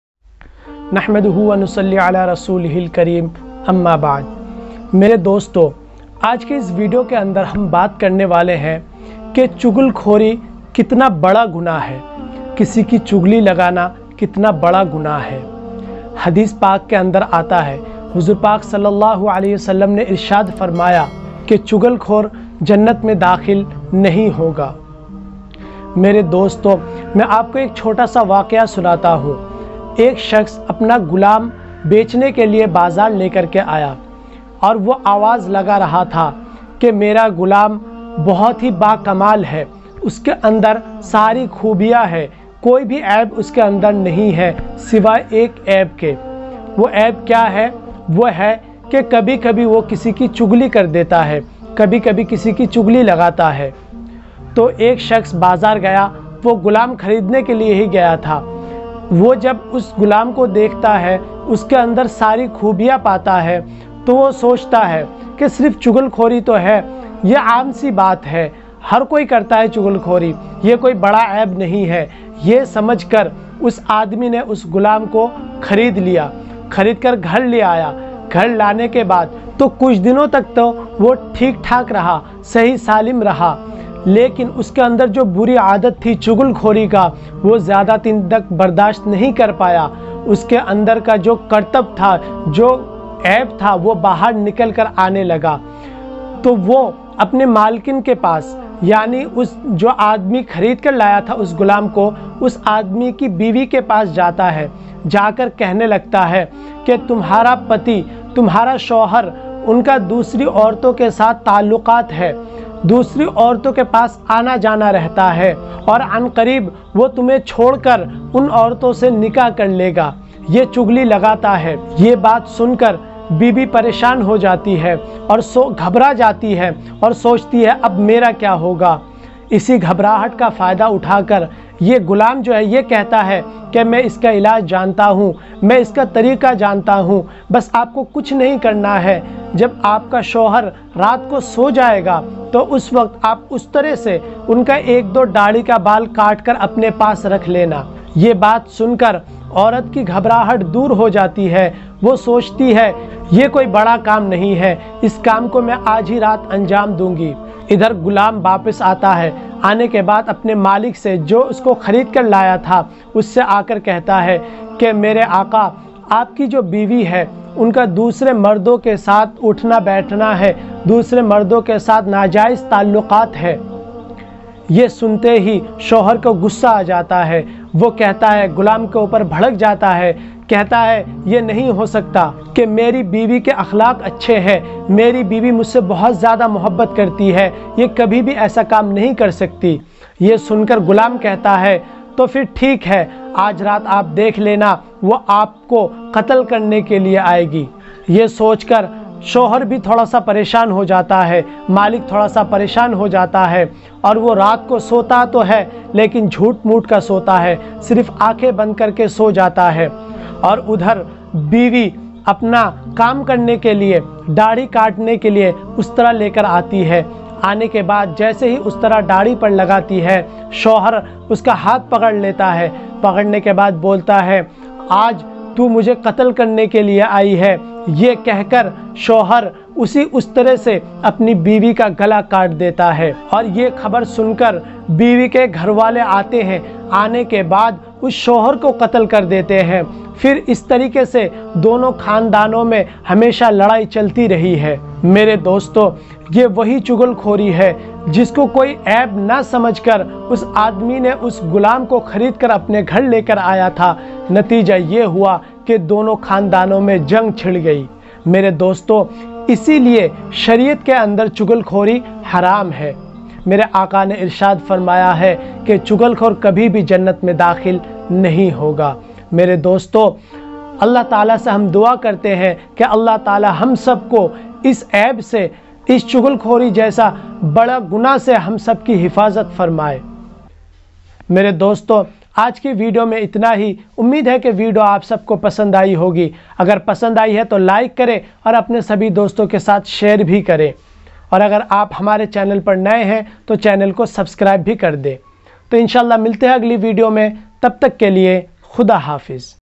Chugli Karne Ka Azab ! एक चुगलखोर का दर्दनाक वाकिया ! चुगली करने वालों की सजा ! Maulana Saidul Islam
is video me ap janenge ke Chugli Karne Ka Azab ! एक चुगलखोर का दर्दनाक वाकिया ! चुगली करने वालों की सजा ! Maulana Saidul Islam